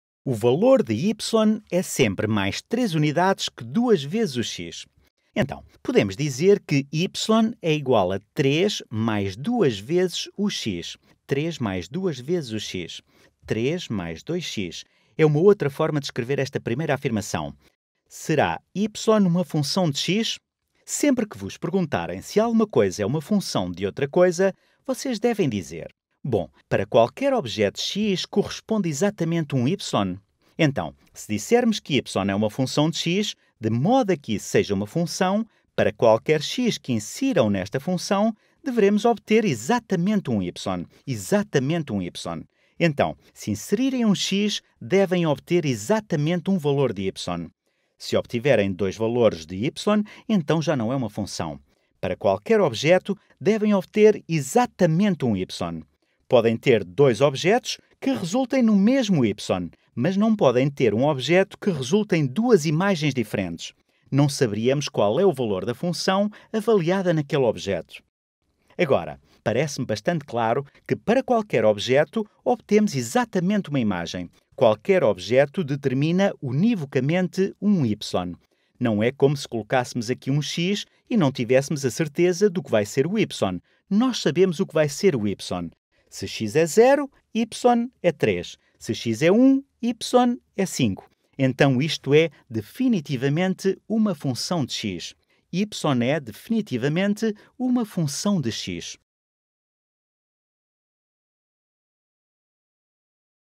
Isto é uma função? (descrição verbal de uma equação)- Khan Academy em português (7º ano)
Vídeo original: Is this a function? (verbal description of equation)